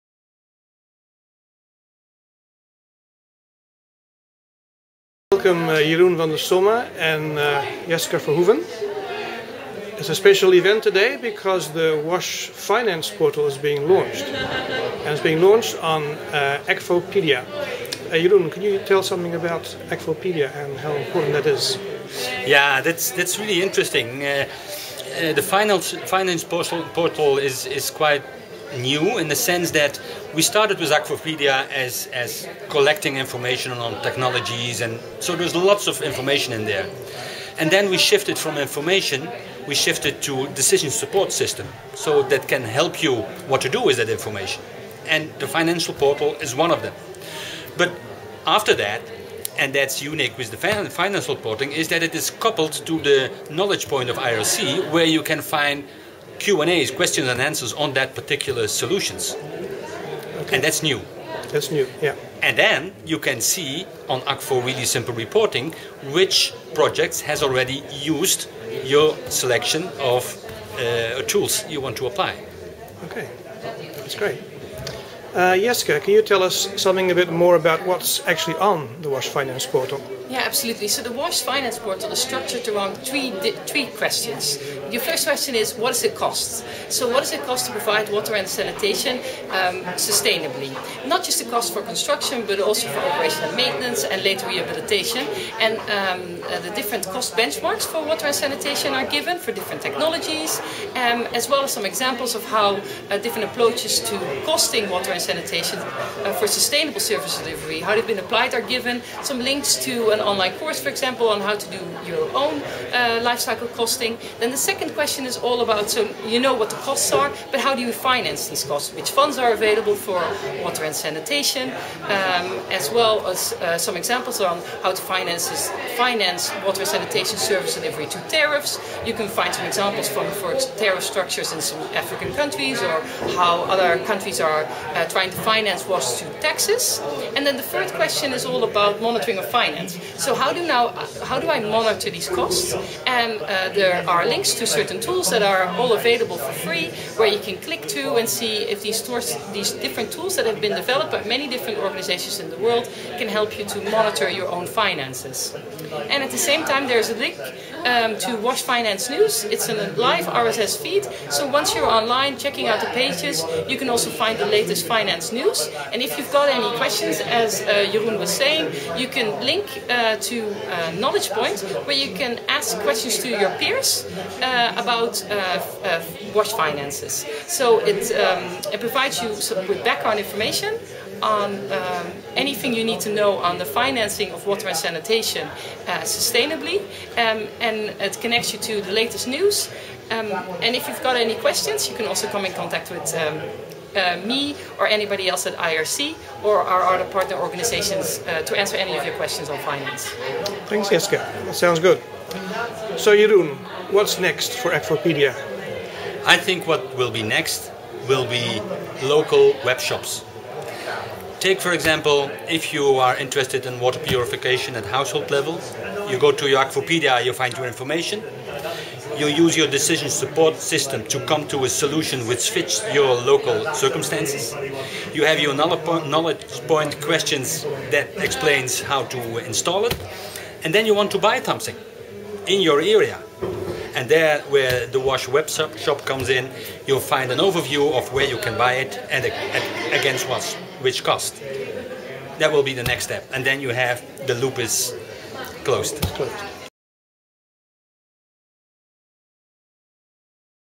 Akvo and IRC introduce the Akvopedia WASH Finance Portal
Akvo's Jeroen van der Sommen and IRC's Jeske Verhoeven officially launch the Akvopedia WASH Finance portal on the WaterCouch during the multi-stakeholder dialogue on water on 21 March 2013.

Interview by IRC's Cor Dietvorst